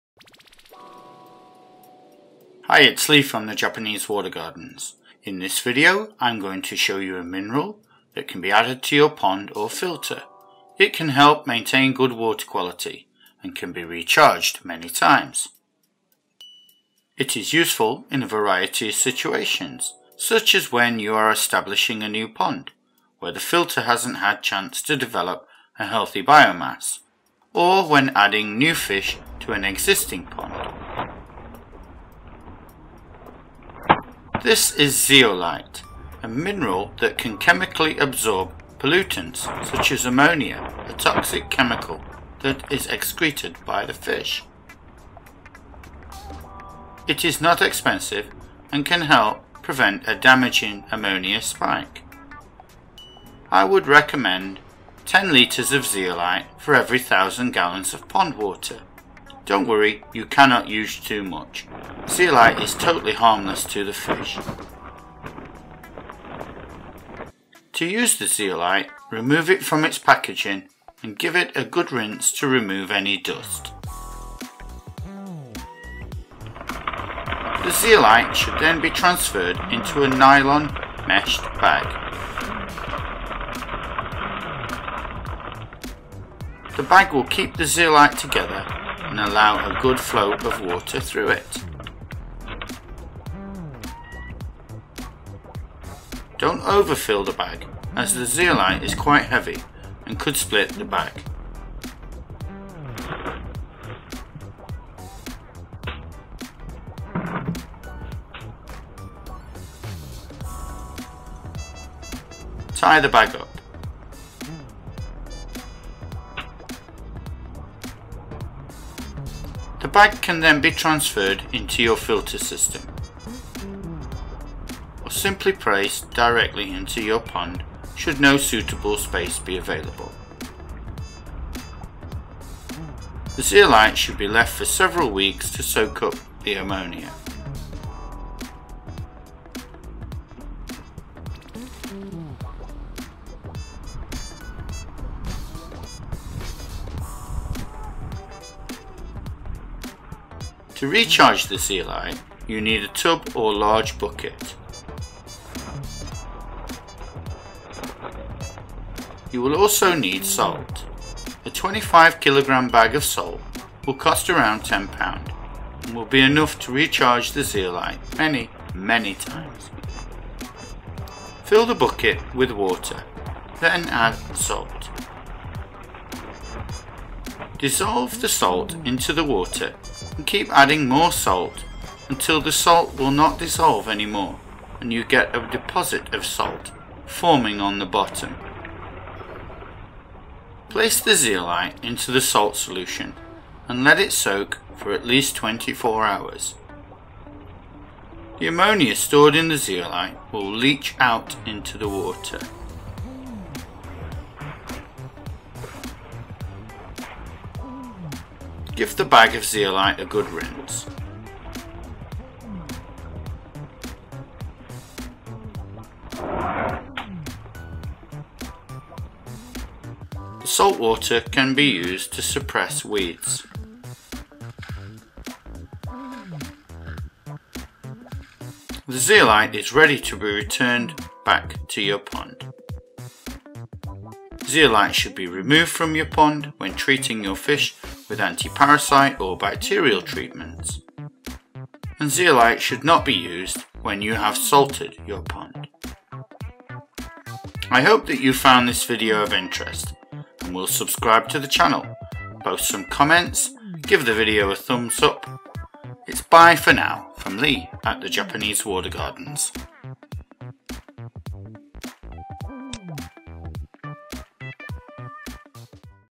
Amazing Mineral That Absorbs Ammonia.
In this video I am going to show you a mineral that can be added to your pond or filter. It can help maintain good water quality and can be recharged many times.

It  is useful in a variety of situations. Such as when you are establishing a new pond, where the filter hasn't had a chance to develop a healthy biomass. Or when adding new fish to an existing pond.